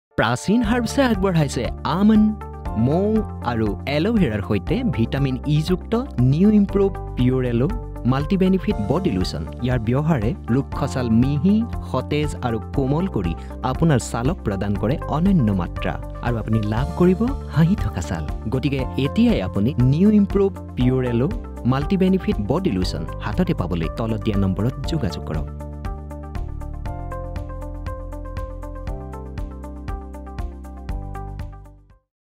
Prachin Herbs New Improved Multi Benefit Body Lotion- in Assamese
Prachin Herbs New Improved Pure Aloe Multi Benefit Body Lotion contains almond and honey, it's full of Vitamin E. Regular use of new improved Pure Aloe Multi Benefit Body Lotion hydrates, moisturises and revives skin, repairs damaged cells, gives you soft and healthy skin by healing and improving the texture.

For hydrated, moisturised soft skin, contact on the number given below and bring home Prachin Herbs made new improved pure Aloe Multi Benefit Body Lotion.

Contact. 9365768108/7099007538